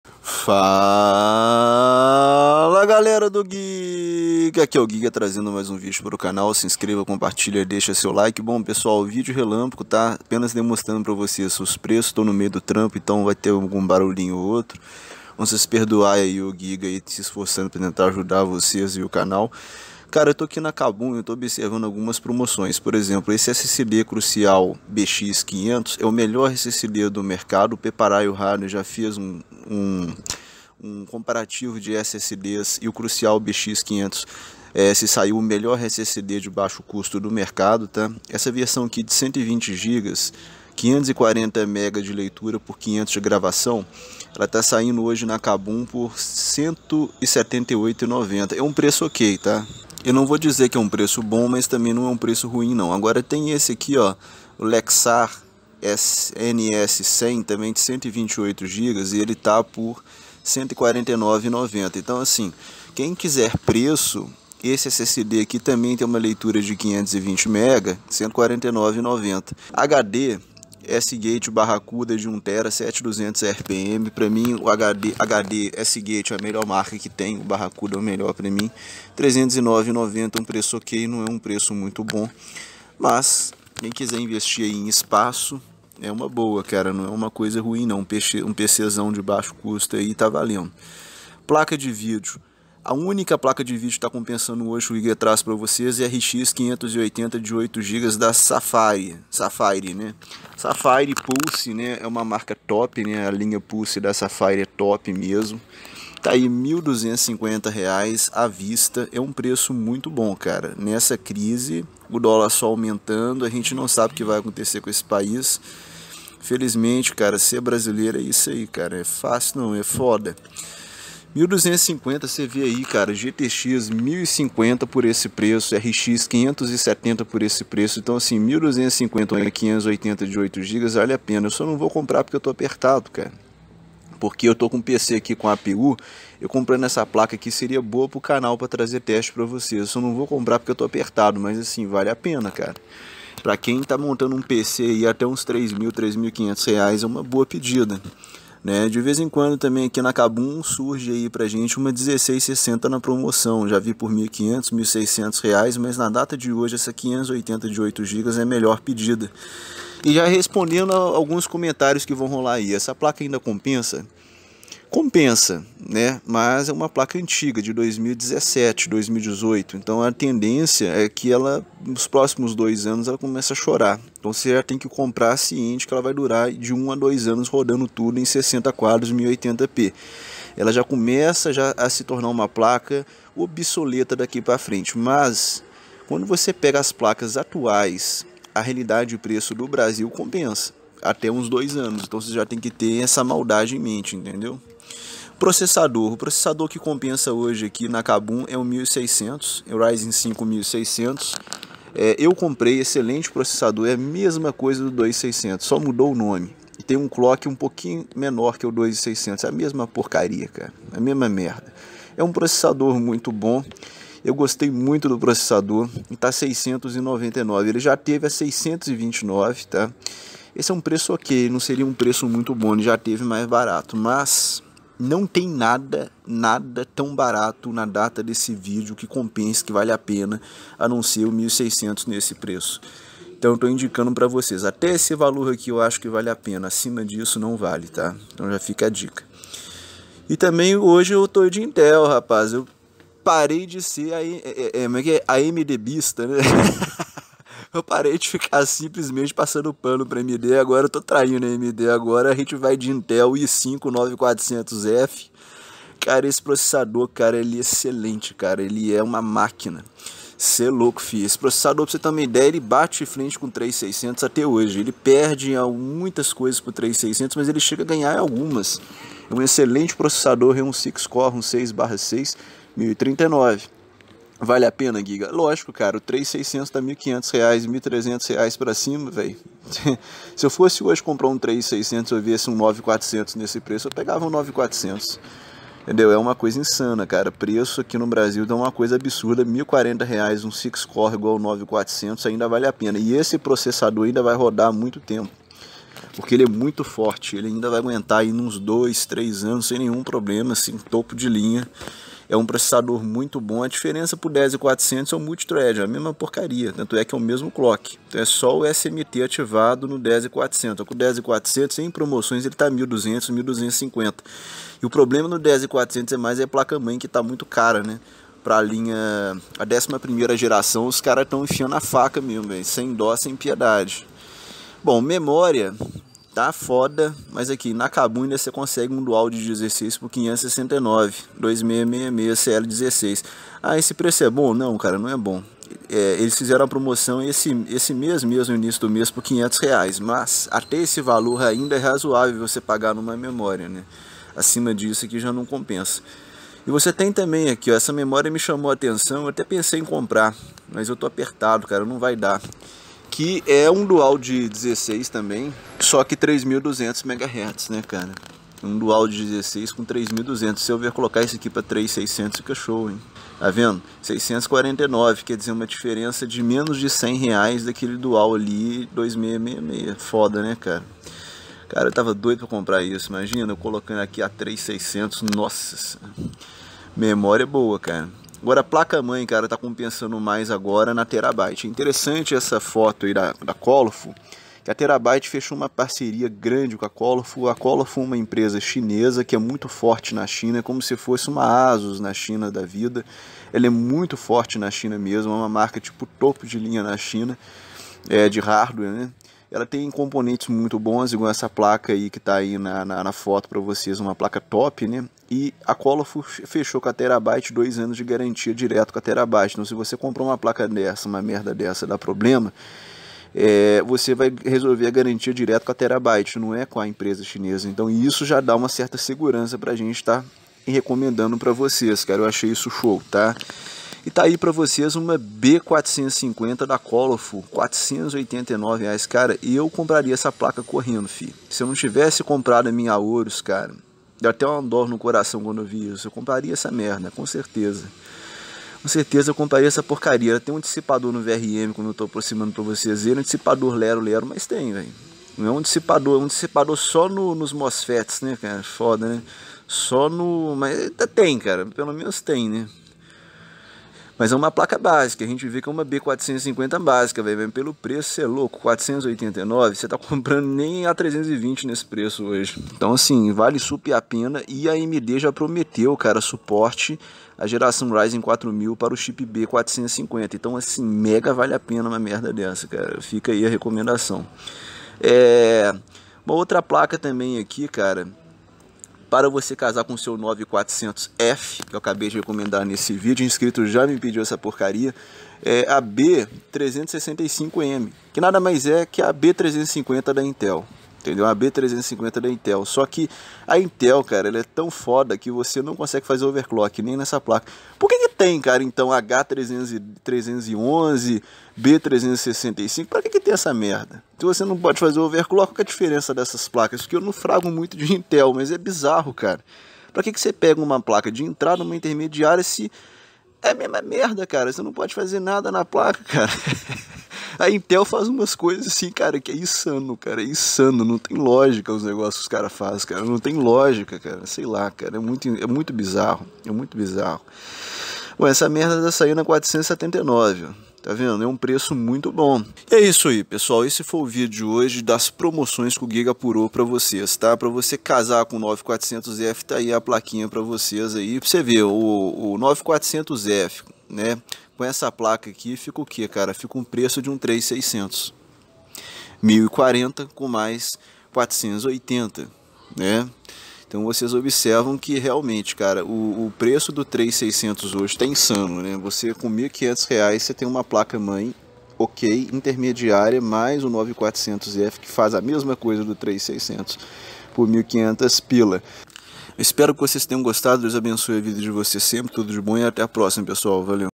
Corraaaaa ! Promoções de Hardware que vale apena hoje 25/08/20 ! PC Barato e Hardware Barato
As promoções de hardware barato de hoje 25/08/20.